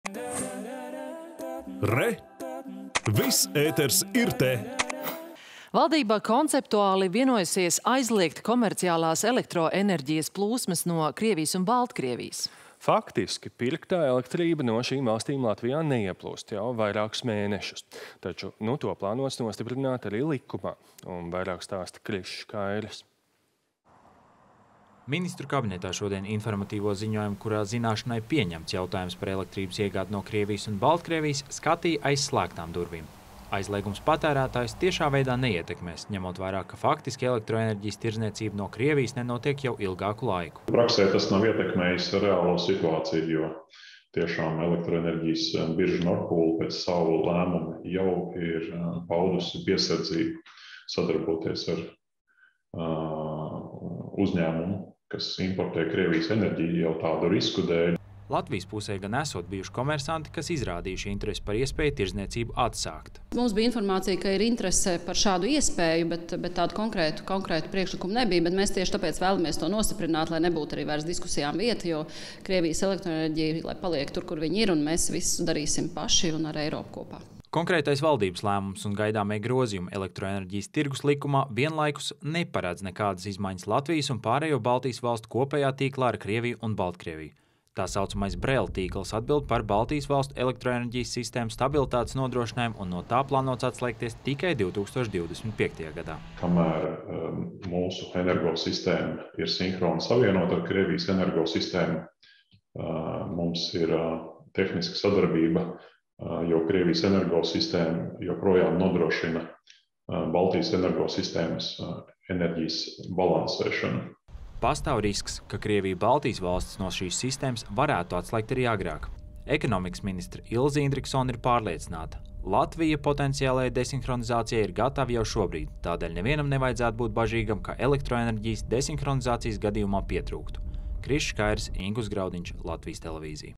Aizliegs komerciālās elektrības plūsmas no Krievijas
Valdība konceptuāli vienojusies aizliegt komerciālās elektroenerģijas plūsmas no Krievijas un Baltkrievijas.
Faktiski pirktā elektrība no šīm valstīm Latvijā neieplūst jau vairākus mēnešus, taču nu to plānots nostiprināt arī likumā. Vairāk stāsta Krišs Kairis.